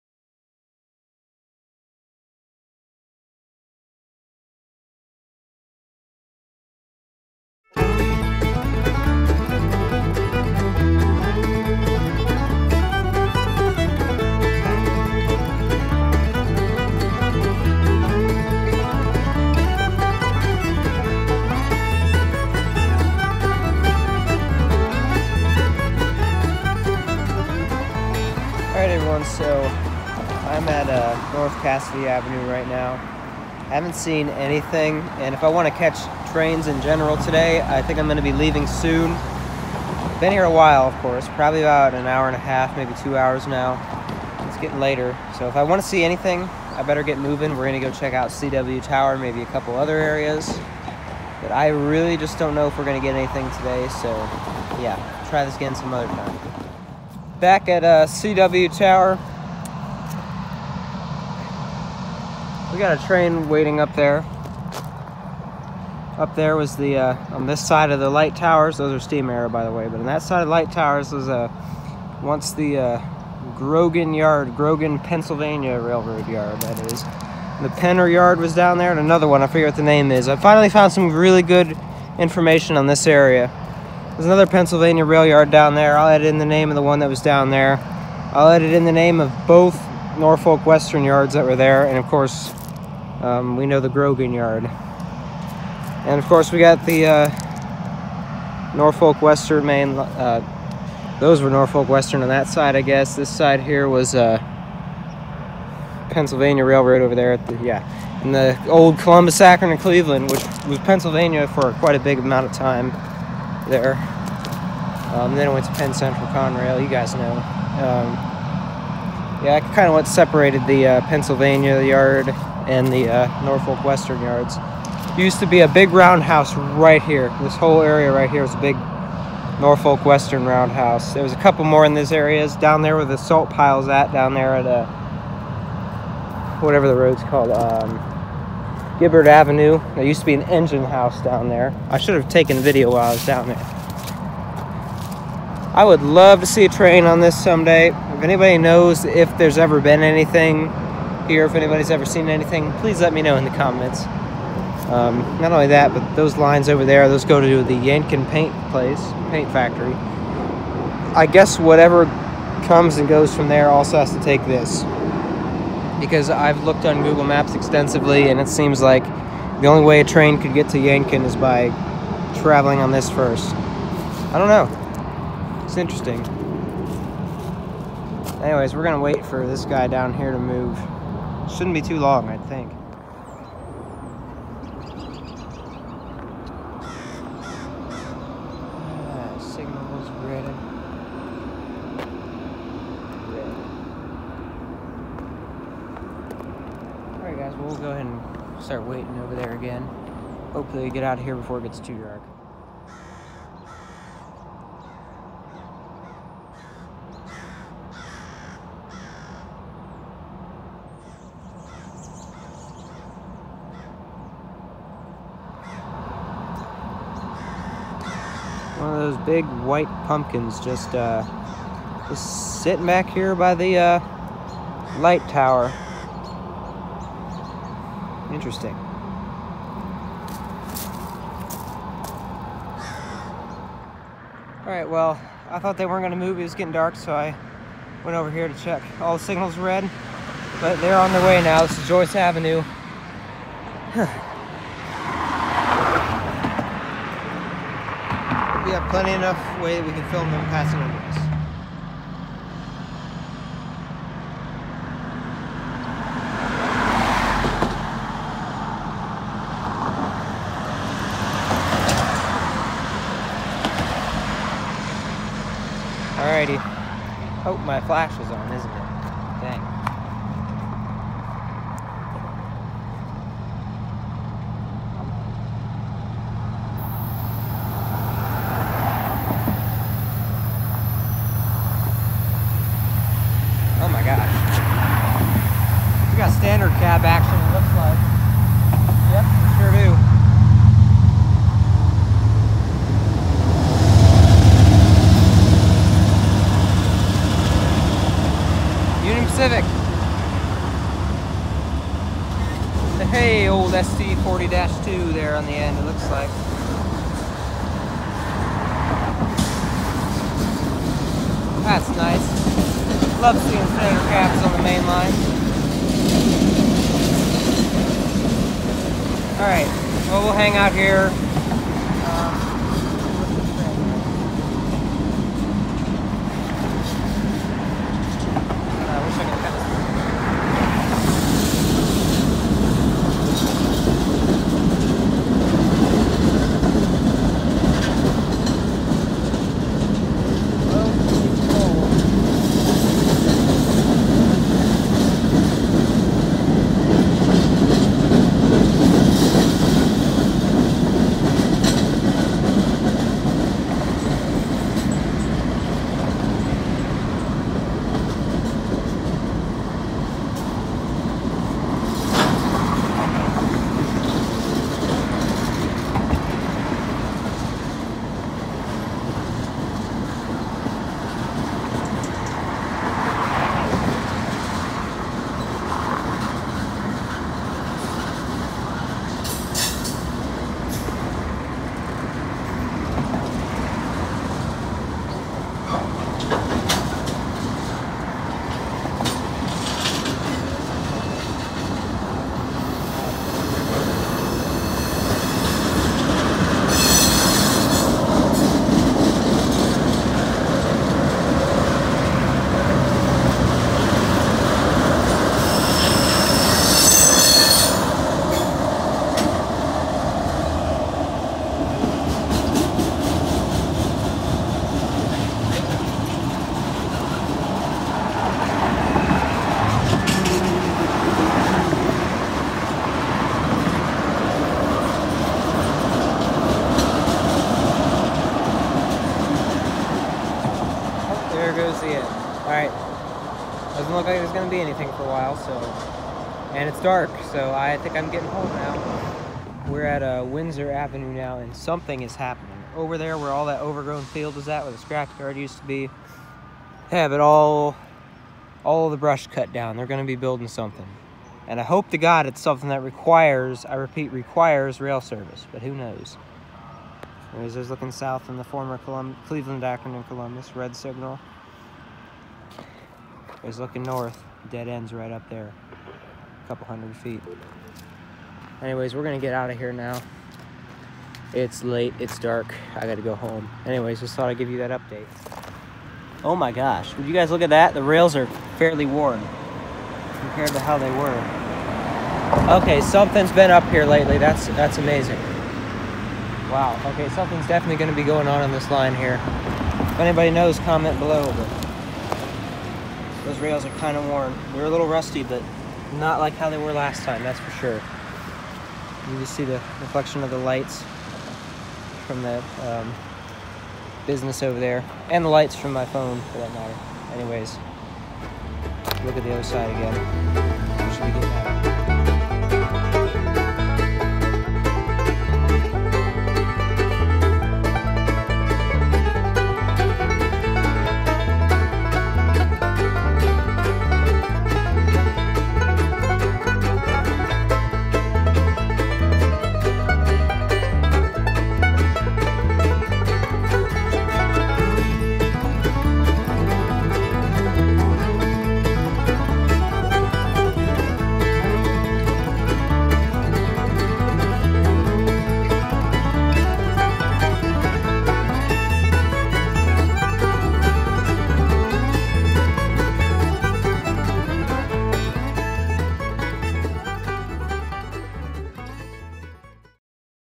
NS Manifest with Foreign power and standard cab. Rail fanning at CW tower
All all footage was captured on November 1, 2022 on that day I had the pleasure of returning once more to CW tower where I only caught a little action and discussed some of the history of the surrounding area.